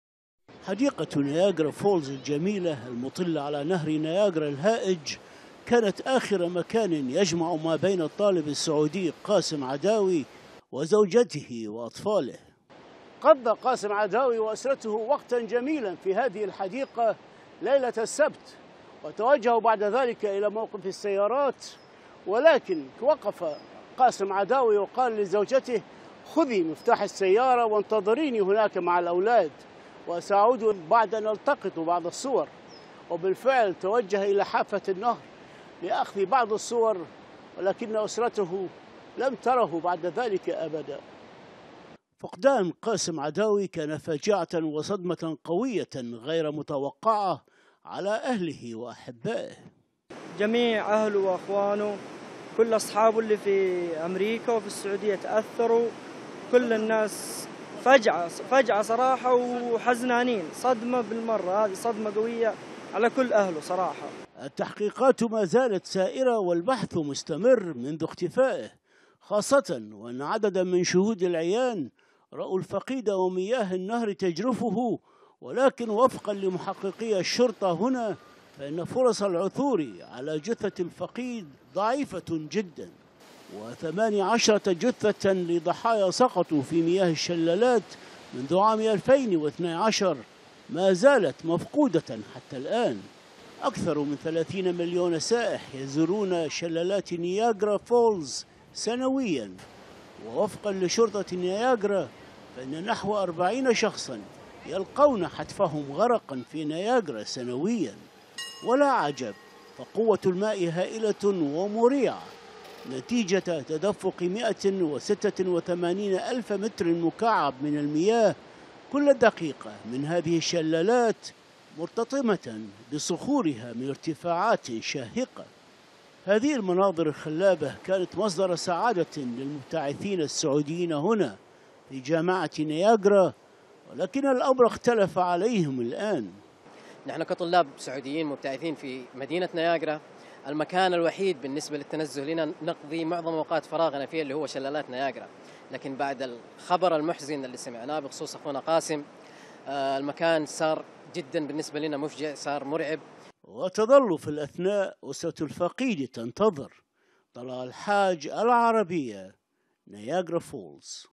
ىحزن في شلالات نياغرا بعد فقدان يحيي
المبتعث السعودي قاسم يحيى عداوي فقد في شلالات نياغرا أثناء رحلة عائلية التحقيقات متواصلة. والحزن يخيم على المكان